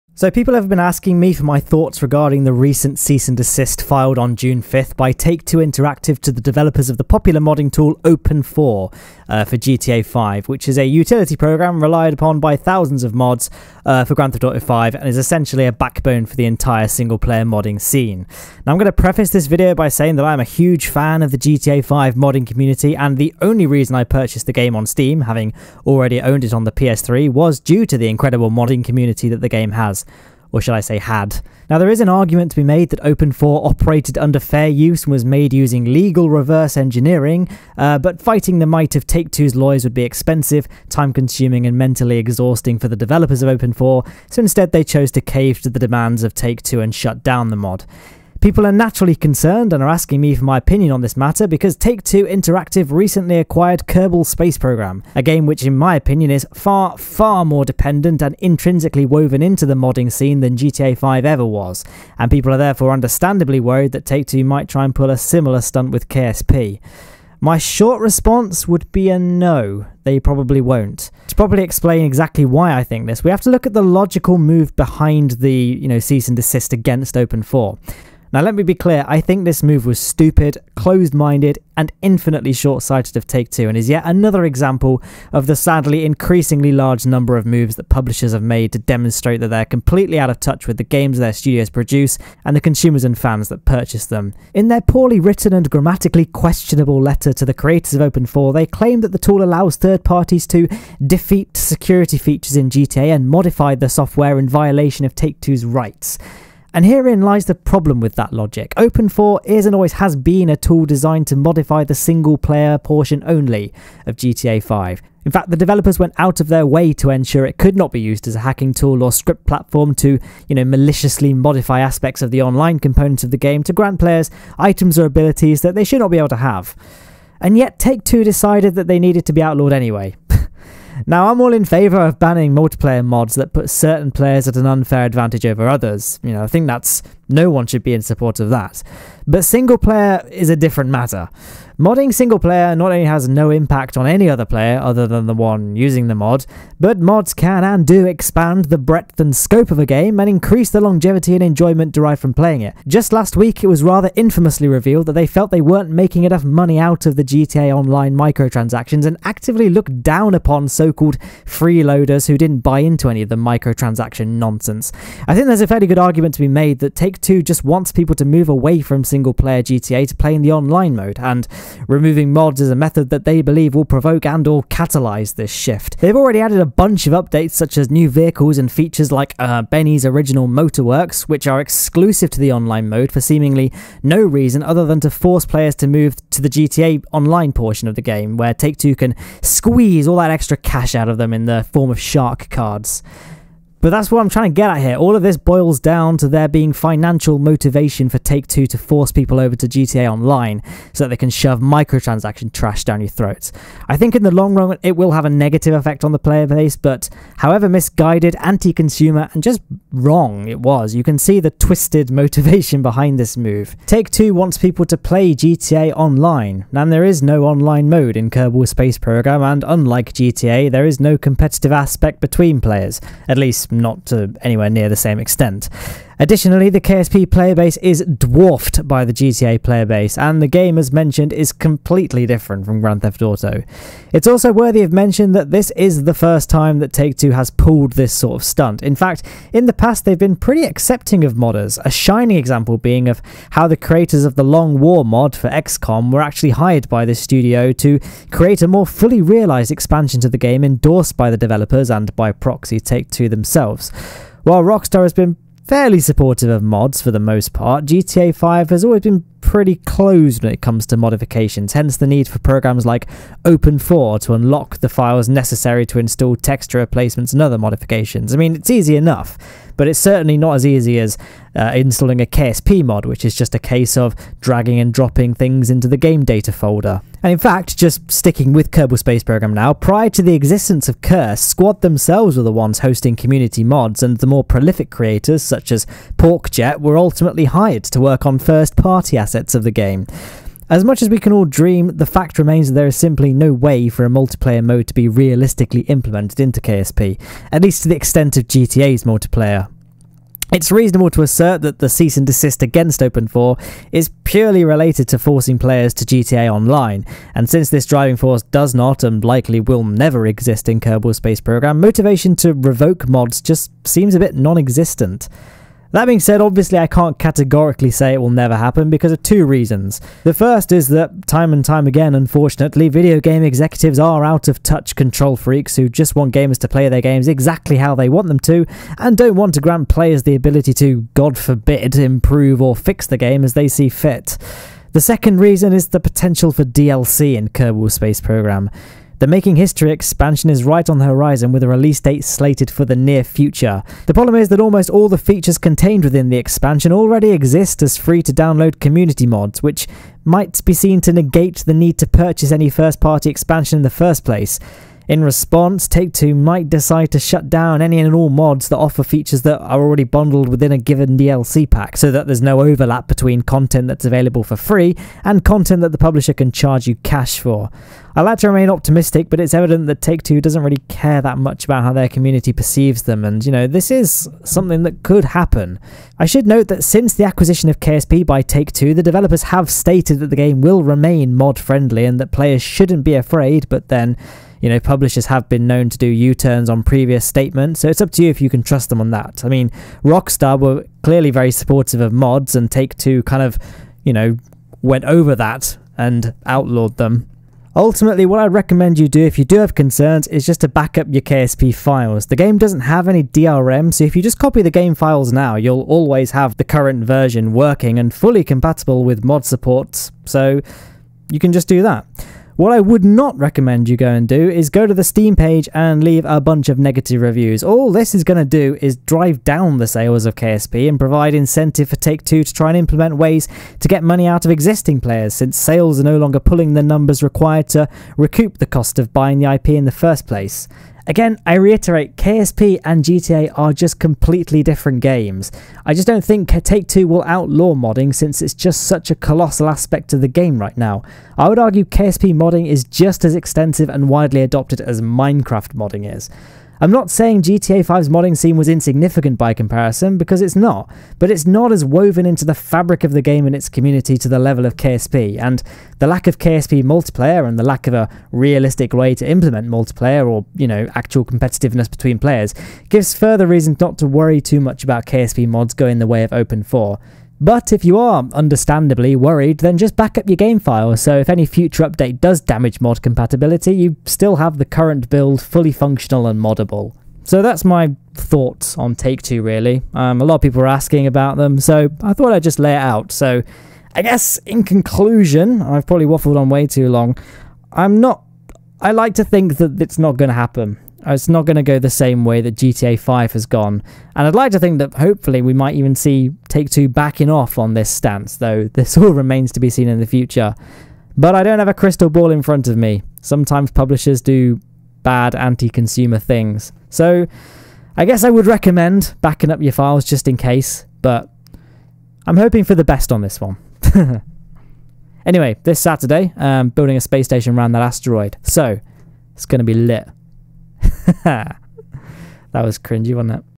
Take-Two and the future of mods for Kerbal Space Program
By popular demand I discuss my thoughts and opinions regarding the whole See below Twitter and other links!

The craft in the video is a prototype and not the finished version of the SSTO, and is therefore not available for download. I will be filing a cease and desist if you reverse engineer it (topical humour!)